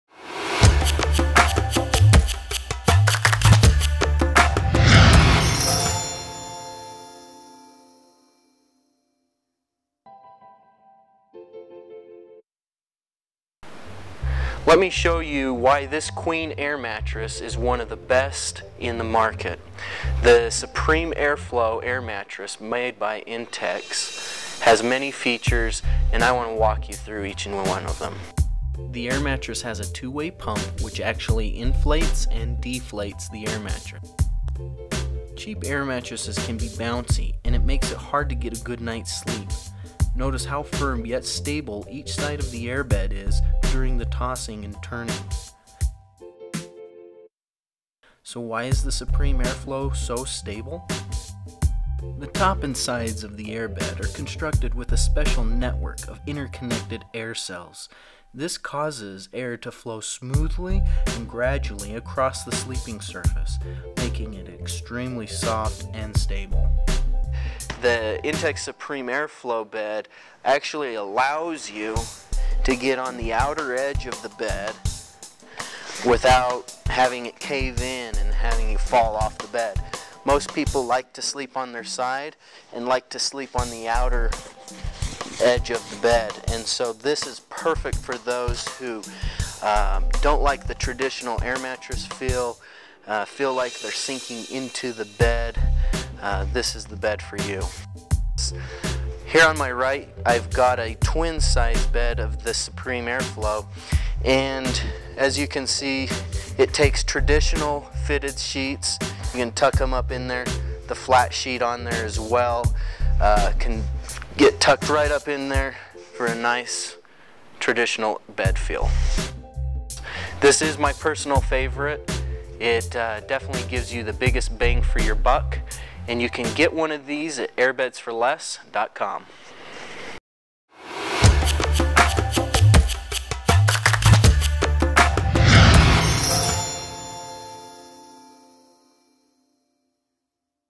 Air Bed
introduces the Intex Supreme Air Flow air mattress. It is ranked above most queen air beds in the market.  The unique air flow structure allows this air bed to be the most firm and stable air mattress out there.